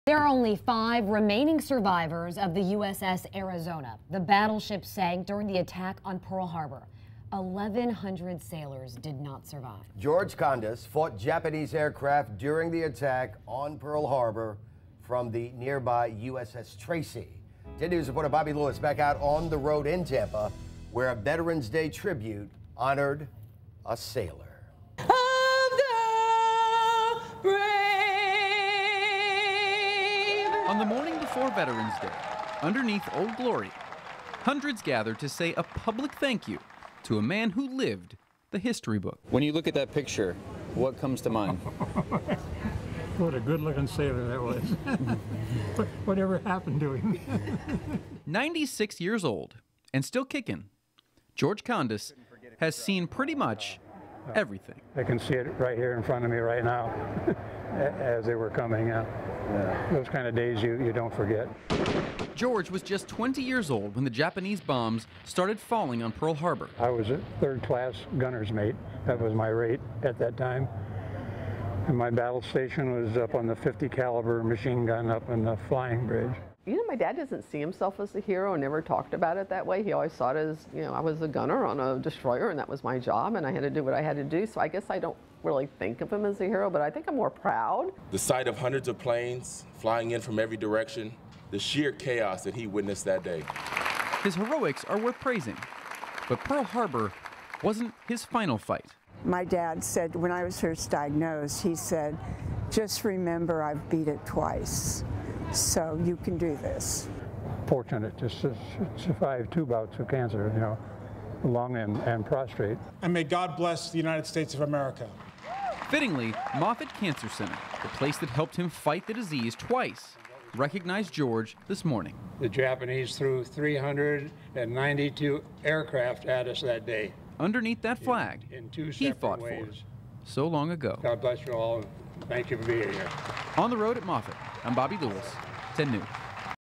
Pearl Harbor sailor honored at Tampa Veteran's Day event
One of the five remaining survivors of the attack on Pearl Harbor lives in Tampa. A Veteran's Day tribute honored the former Navy sailor.